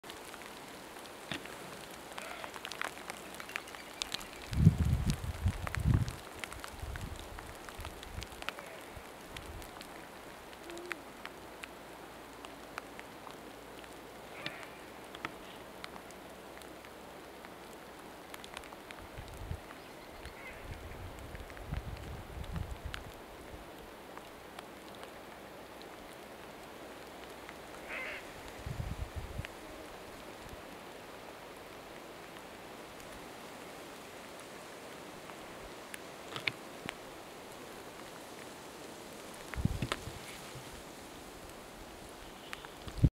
Huuhkajapari juttelee hetken kesäyössä.
Pikku kesäsade ei huuhkajia haitannut.

Pori 3.6.2015